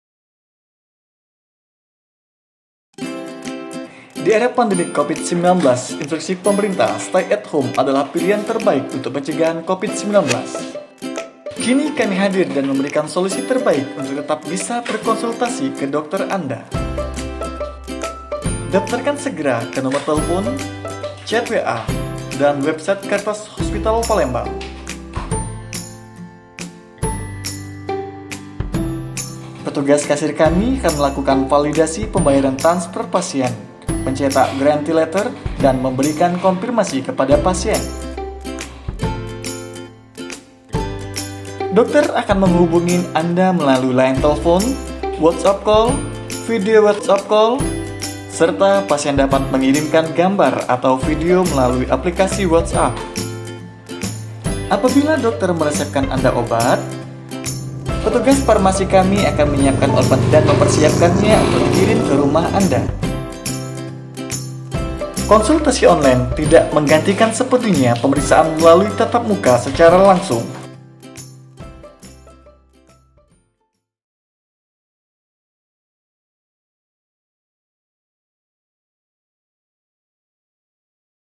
Konsultasi Online di Charitas Hospital Palembang
Mau berobat tapi gak mau keluar rumah ? Konsultasi Online aja.
Daftarkan melalui line telpon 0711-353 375 ext 296 / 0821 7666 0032
atau Chat Whatsapp di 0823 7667.